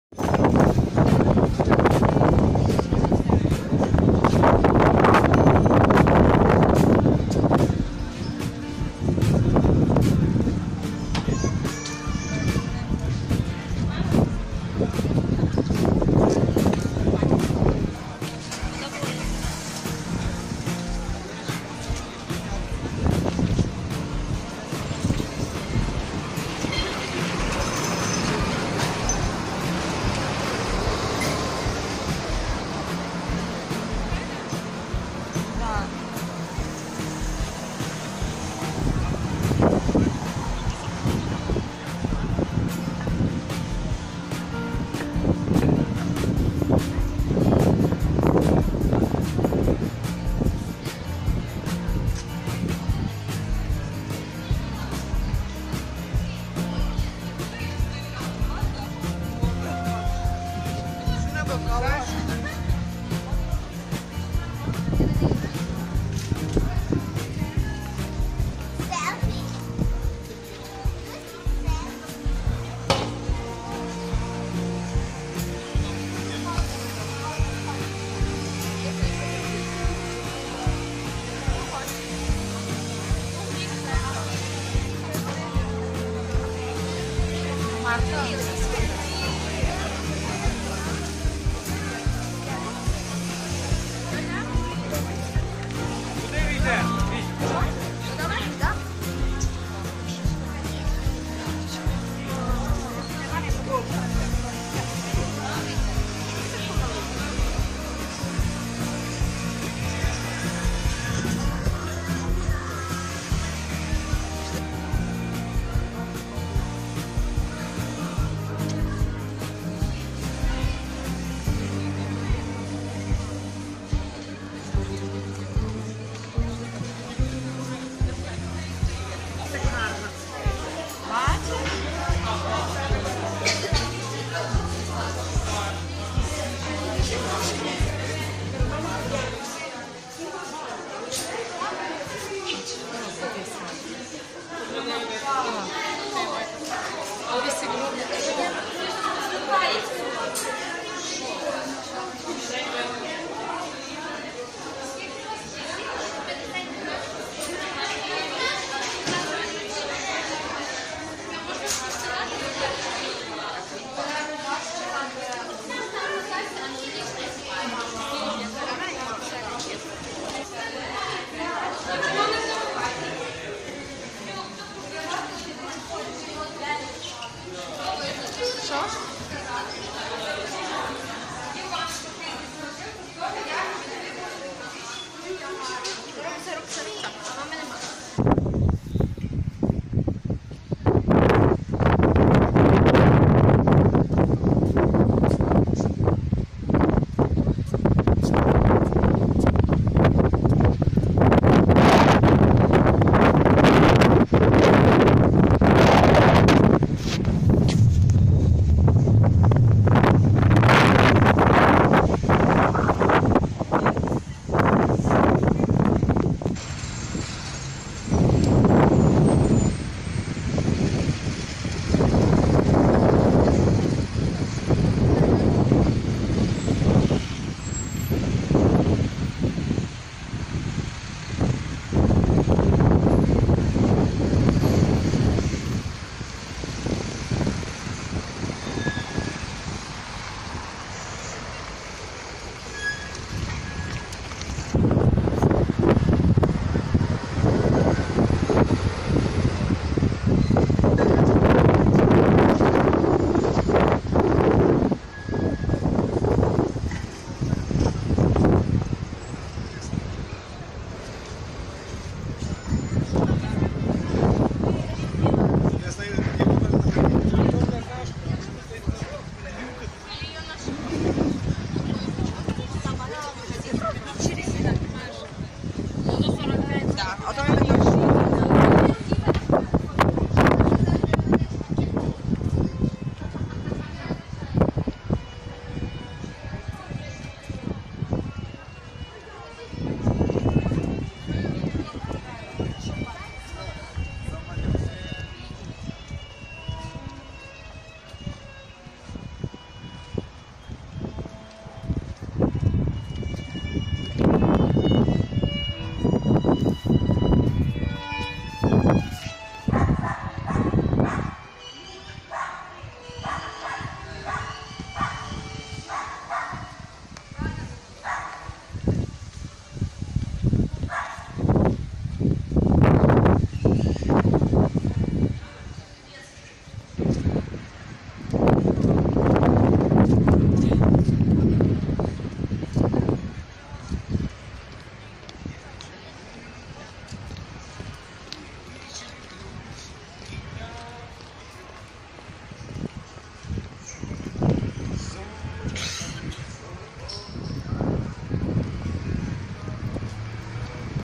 Львів Lviv Lvov 2019
прогулянка центральною частиною Львова